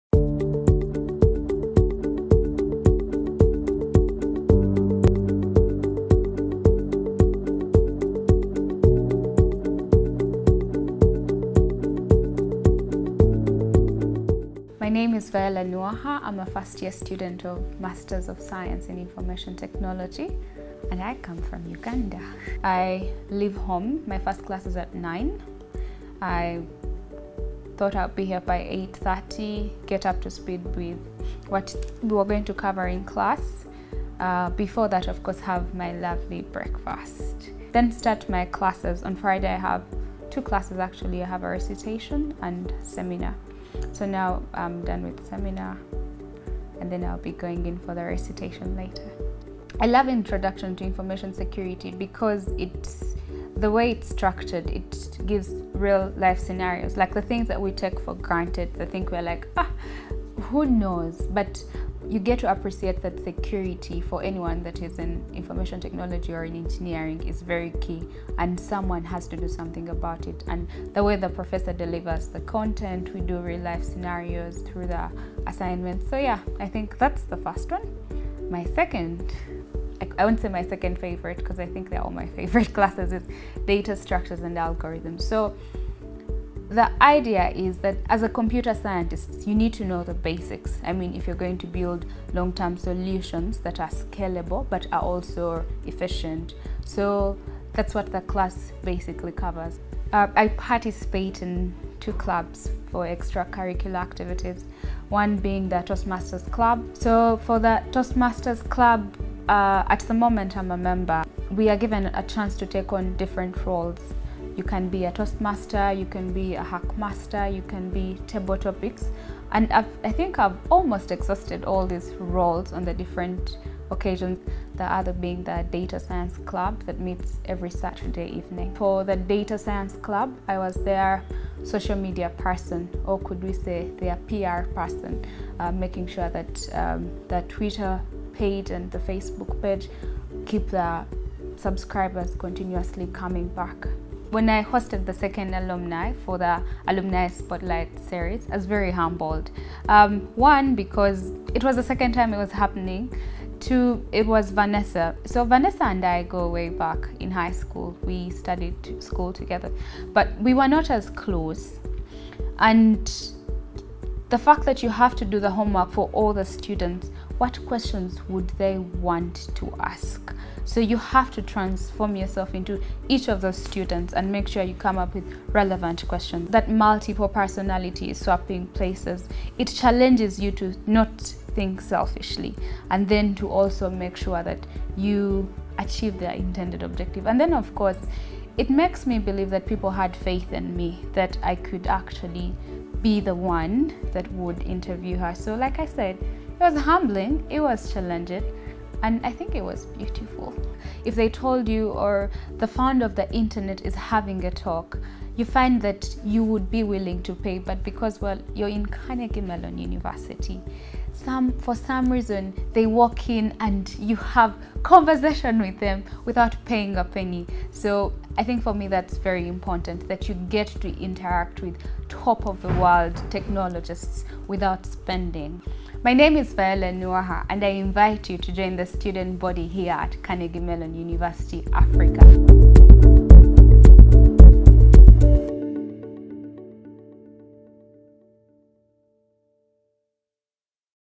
A Day in the Life of Student: Viola Nuwaha
Viola Nuwaha from Uganda walks us through a day in her life as a CMU-Africa student.